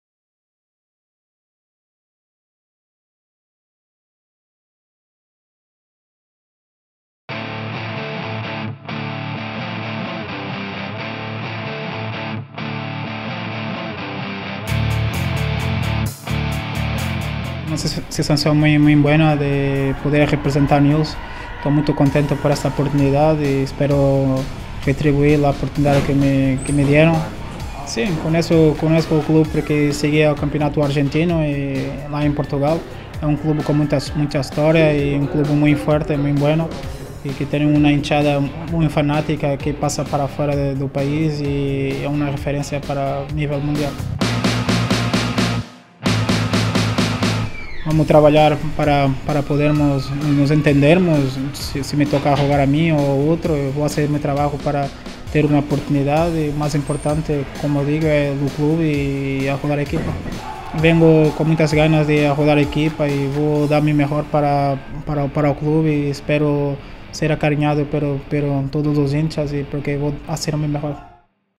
Newell's sumó al delantero Luis Leal
El delantero portugués de 30 años firmó contrato por un año y tras convertirse en nuevo futbolista leproso, afirmó: "Voy a trabajar y a dar lo mejor para tener una chance y poder ayudar al equipo".

Video y edición: Sebastián González.